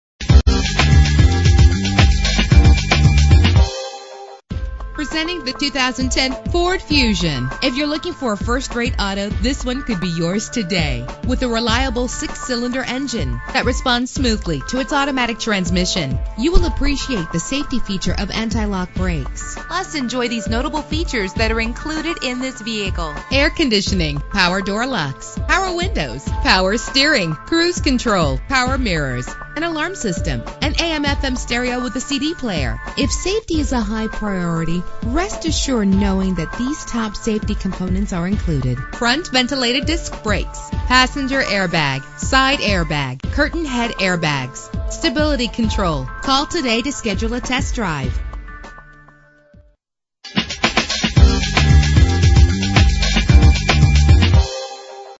2010 Ford Fusion - Rosenberg TX
Year: 2010
Make: Ford
Model: Fusion
Trim: SE
Engine: 3.0 liter 6 cylinder 24 valve
Transmission: Automatic
Color: Black
Mileage: 50639
Address:
27225 Southwest Fwy